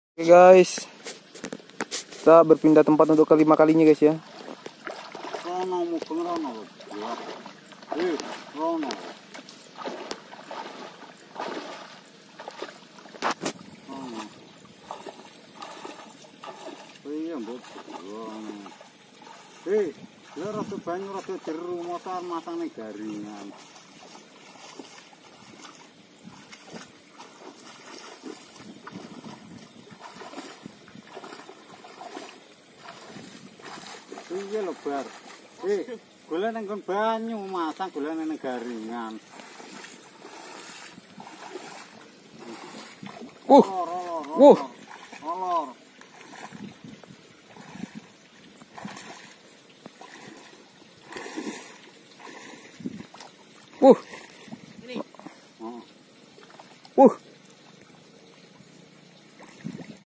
Jaring Ikan Di Empang Rusak Yang Menjadi Sarang Ikan
Menjaring ikan merupakan aktifitas yang sudah sering dilakukan oleh para pemburu ikan, dengan cara tradisional dan tidak mengancam kerusakan lingkungan.

Tolong Bantu Kejar 1000 Subs ya guys, makasih yaa udah bantu